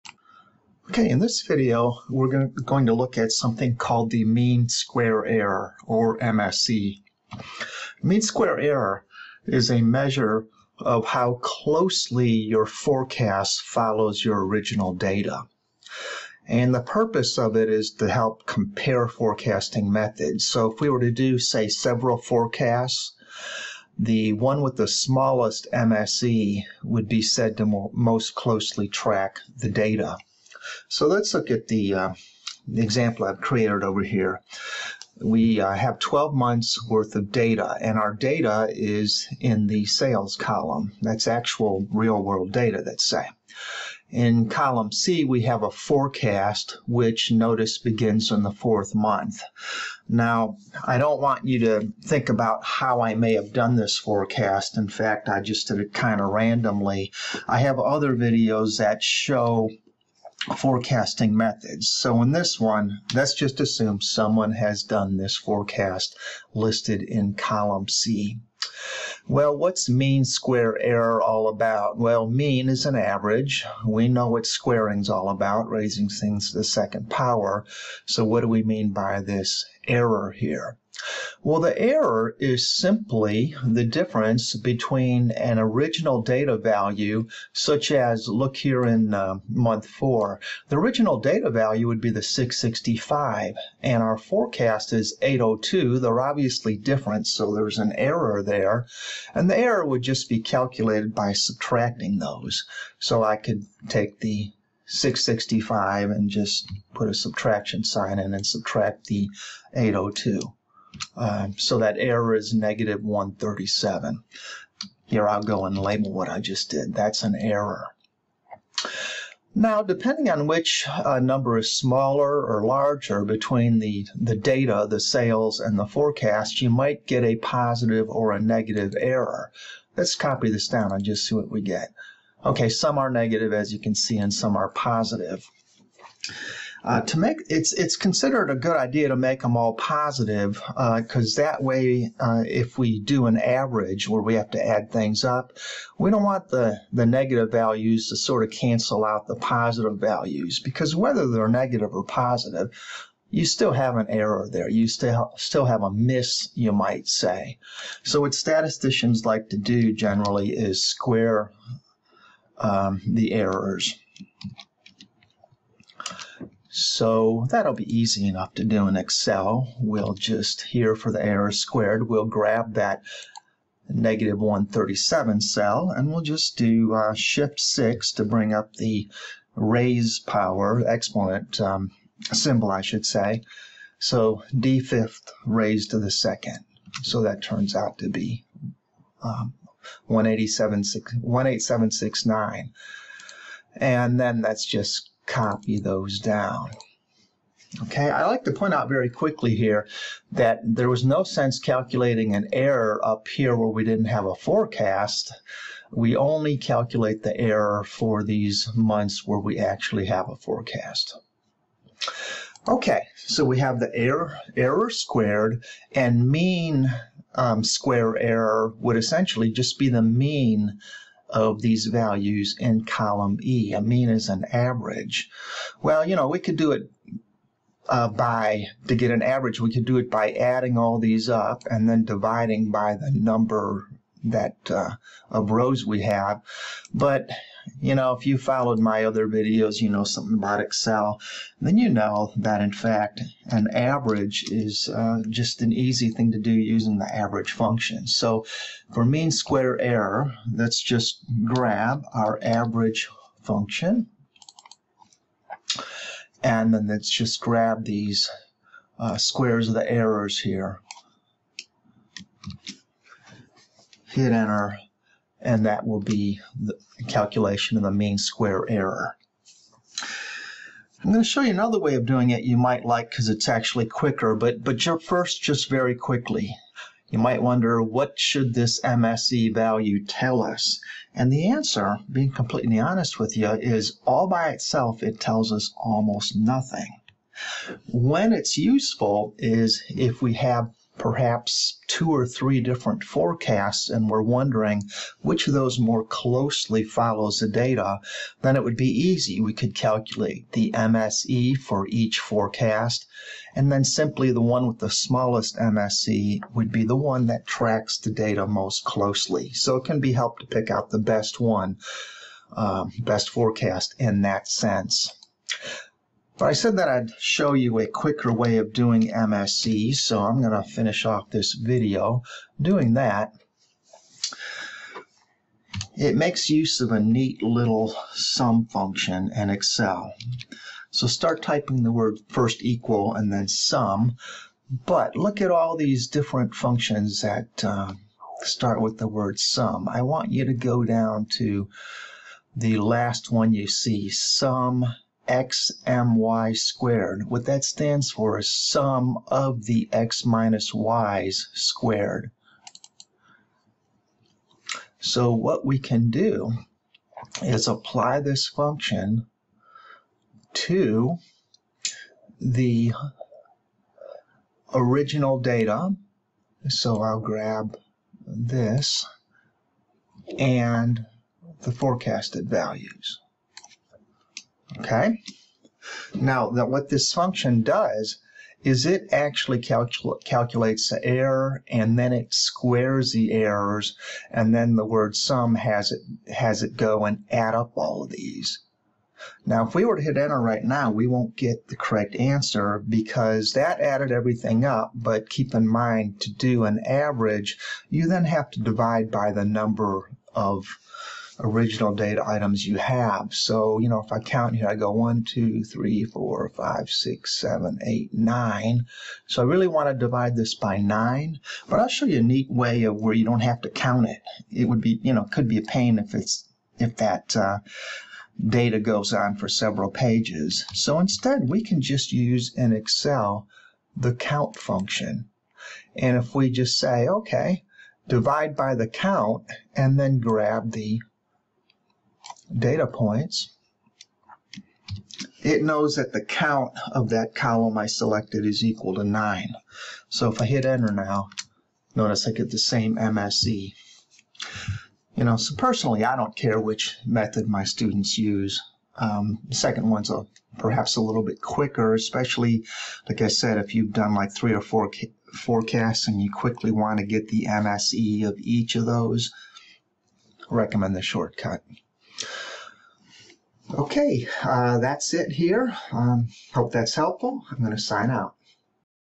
Mean Square Error (MSE)
MSE is used to compare different forecasting methods. If the goal is to closely follow the original data, then the forecasting method with the lower MSE more closely tracks the data.